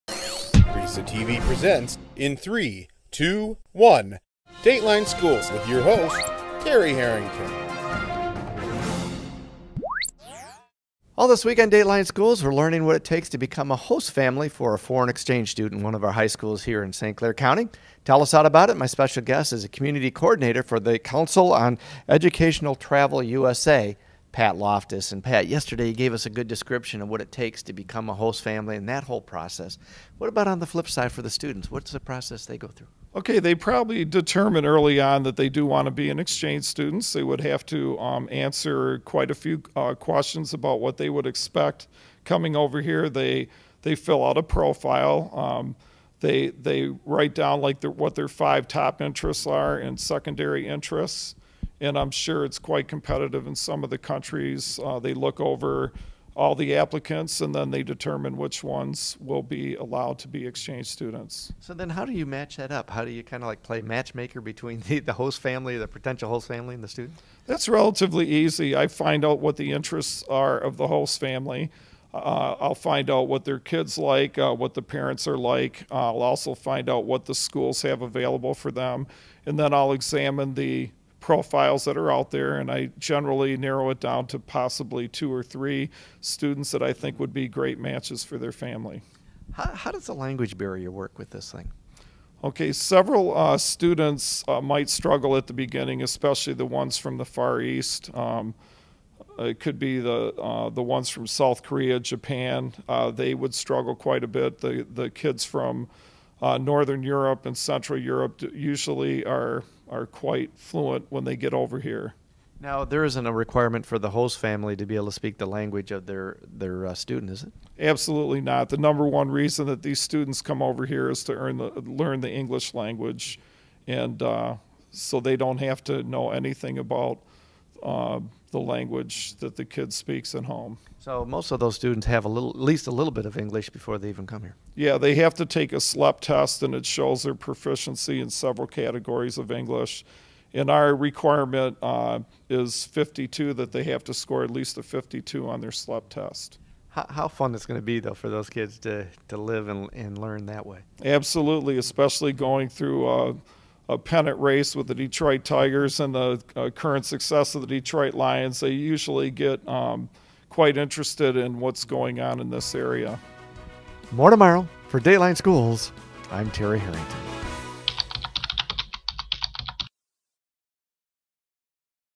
Dateline Schools - Council for Education Travel - Thursday
Patrick Loftus, who works for CETUSA, an organization that places foreign exchange students with local families, talks about the opportunities locally to host foreign students.